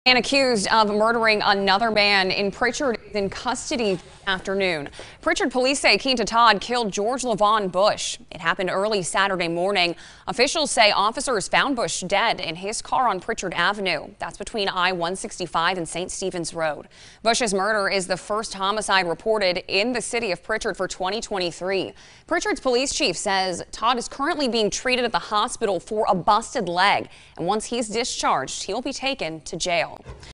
Suspect in first Prichard homicide of 2023 in police custody: Police
Officials with the Prichard Police Department have confirmed to WKRG that the man accused of murdering another man in Prichard is in police custody.